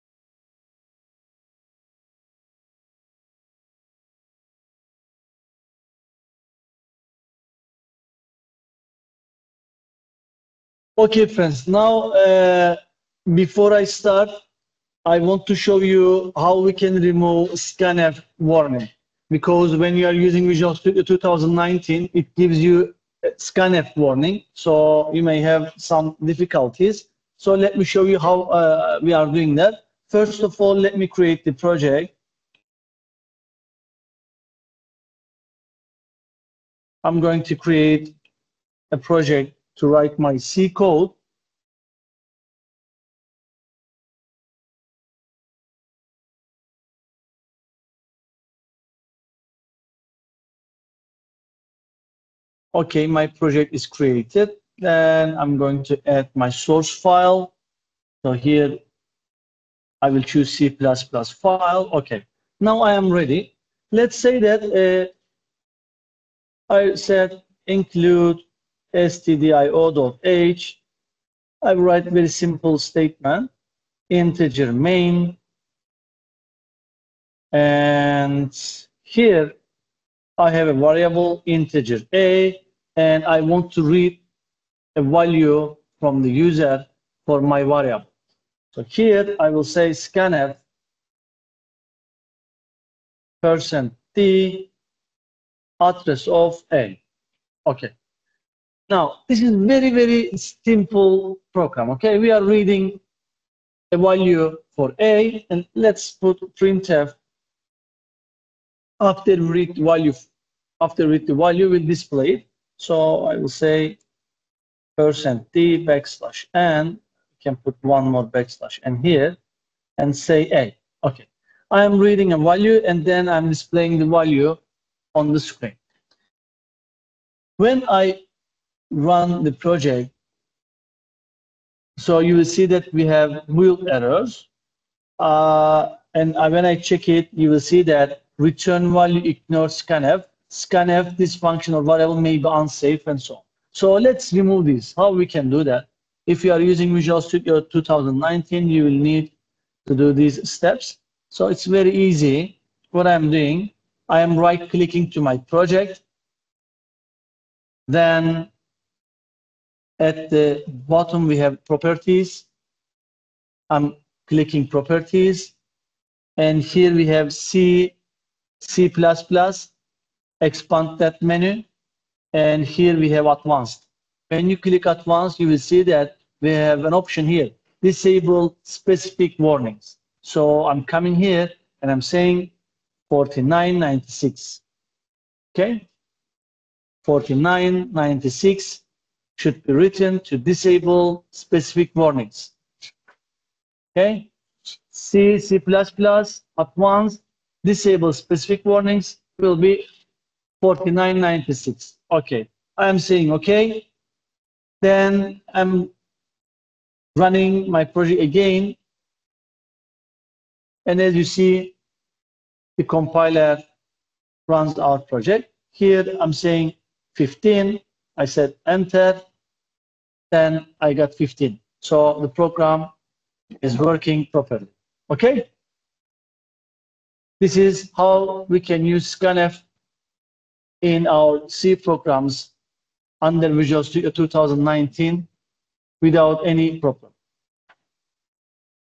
How to remove warning and error for scanf
This is a supplementary material for the C-programming-related courses given by Assoc. Prof. Dr. Yıltan Bitirim in the Computer Engineering Department of Eastern Mediterranean University.